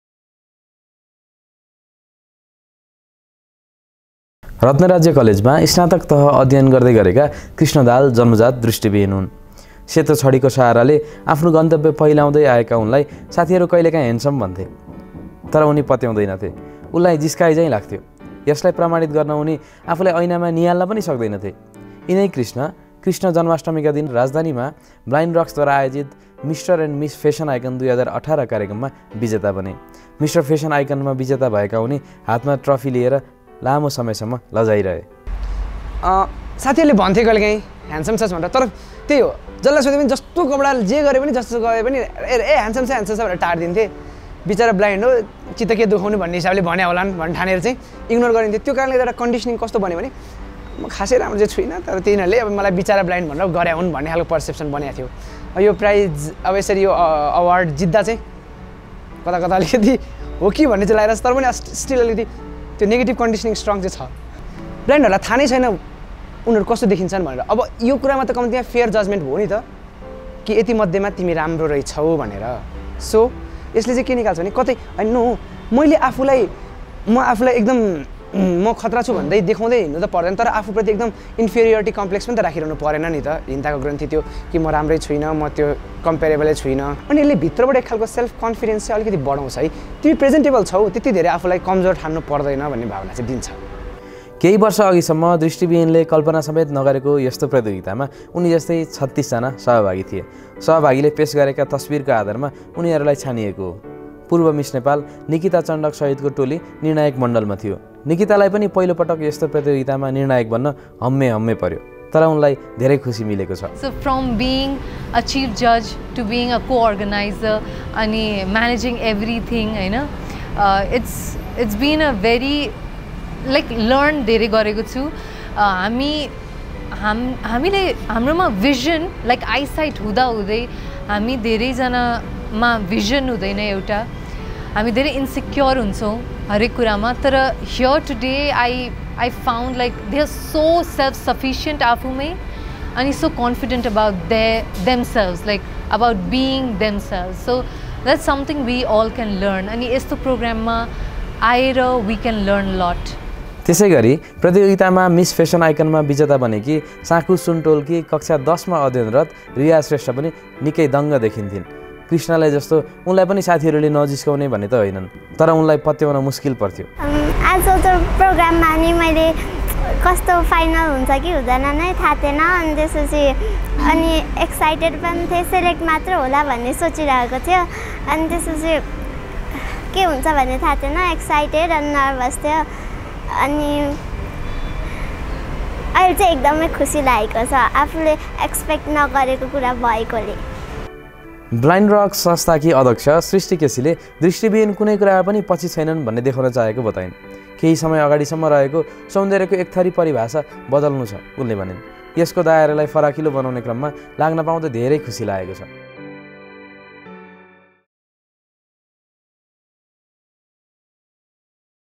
दृष्टिविहीनको सौन्दर्य प्रतियोगिता
दृष्टिविहीन कृष्ण दाहाल कृष्णअष्टमीका दिन आइतबार राजधानीमा आयोजित  कार्यक्रममा ‘मिस्टर फेसन आइकन’मा विजेता बन्दा निकै समय लजाए । ‘मलाई म राम्रो छु भन्ने कहिल्यै विश्वास लागेको थिएन,’ ब्लाइन्ड रक्सले आयोजना गरेको मिस्टर एण्ड मिस फेसन आइकन प्रतियोगितामा विजयी उनले भने, ‘मेरो लागि यो कार्यक्रम  आफैलाई नियाल्ने ऐना बन्न पुगेको छ।’

Story-Upendra Lamichhane

Video/Edit-Dipendra rokka